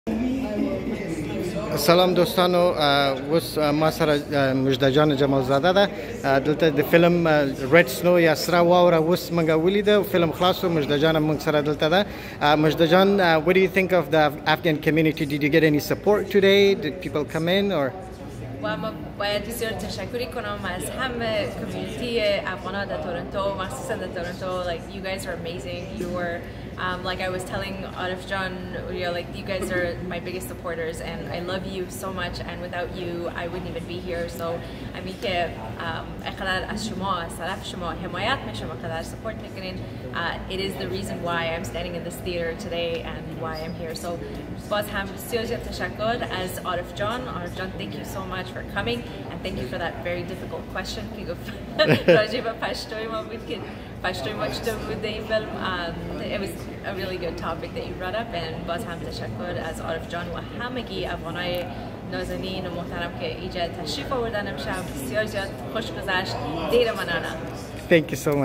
Mozhdah Jamalzada || مژده جمال زاده
Arif Orya's interview with Afghan female singer and Hollywood actress Mozhdah Jamalzada at TIFF after the premiere of her movie ( Red Snow ).